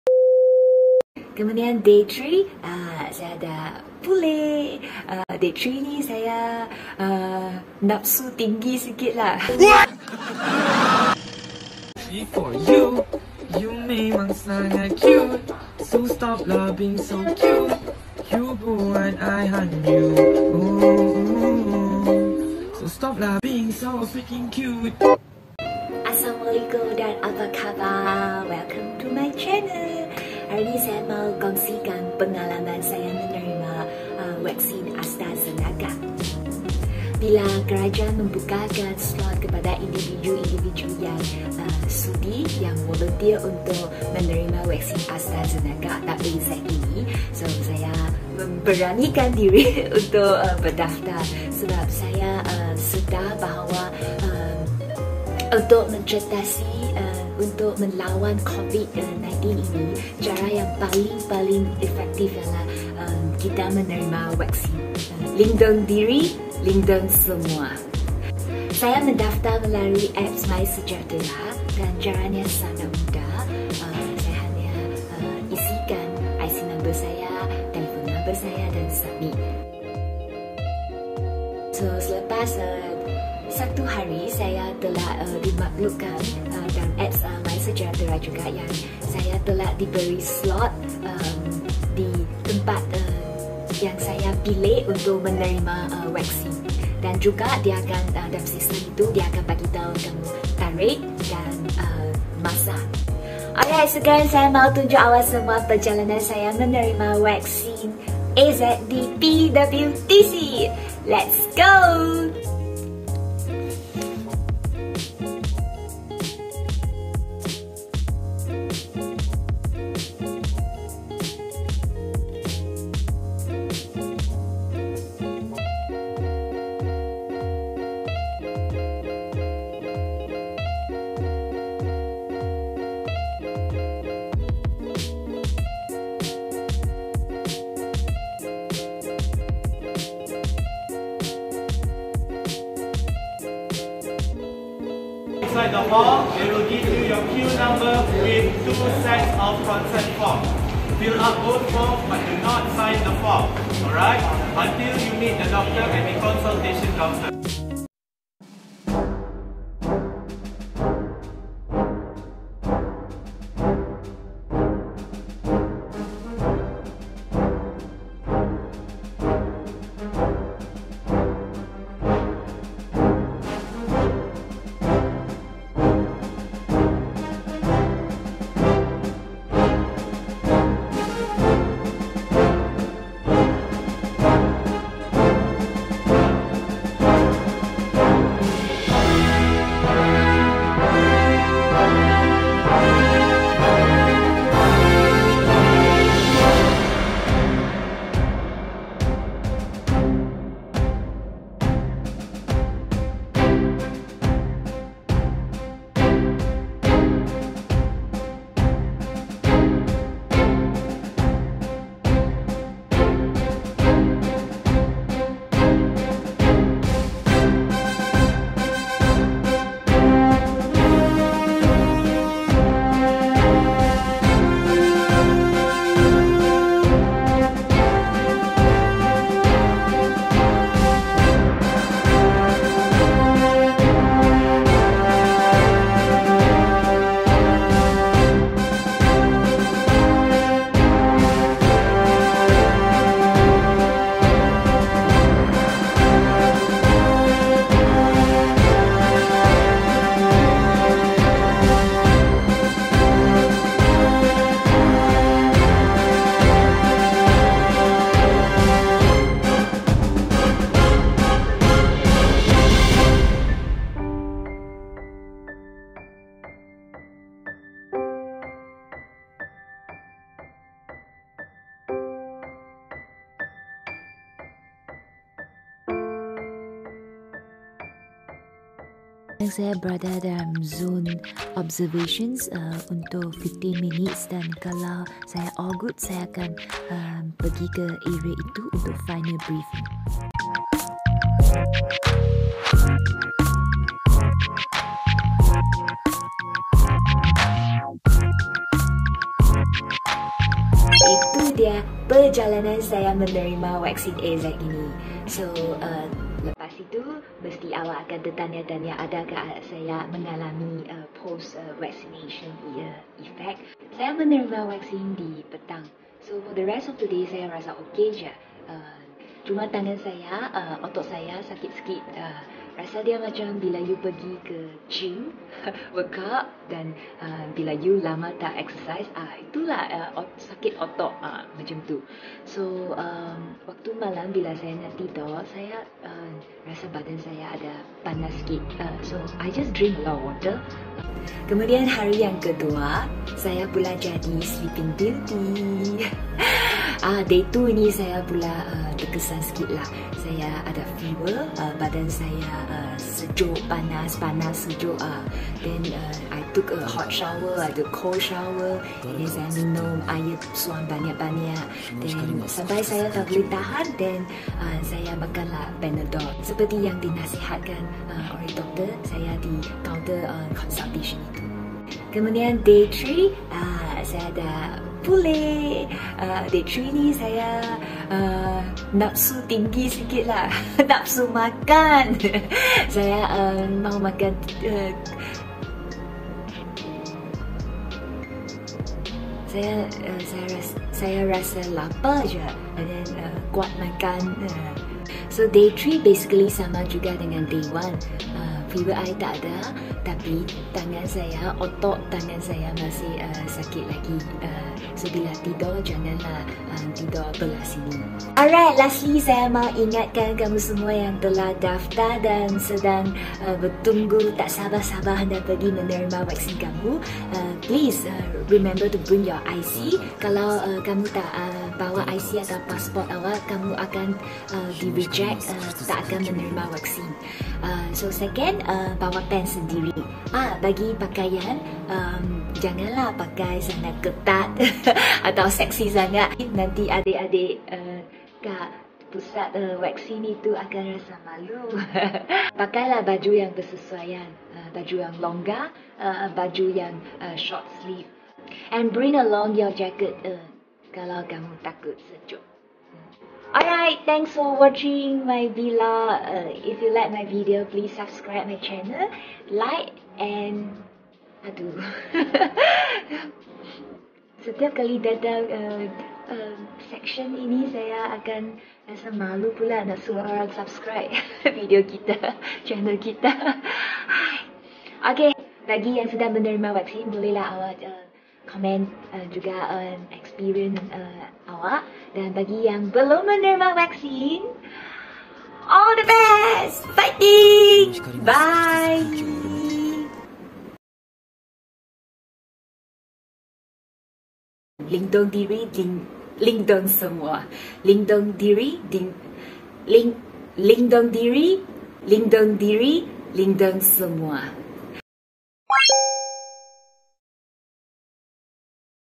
CucukMyAZ | Side Effects AstraZeneca vaksin | 我打了AZ疫苗, 72小时纪录 | 自愿接种阿斯利康疫苗
AstraZeneca vaccine jab from A to Z  experience at World Trade Centre Kuala Lumpur
Proses dari A to Z, cucuk vaksin AZ dos petama kat World Trade Centre KL.

👍 喜欢我的影片，记得帮我【分享】和【Like】谢谢大家～

Shot & Edit by limmeifen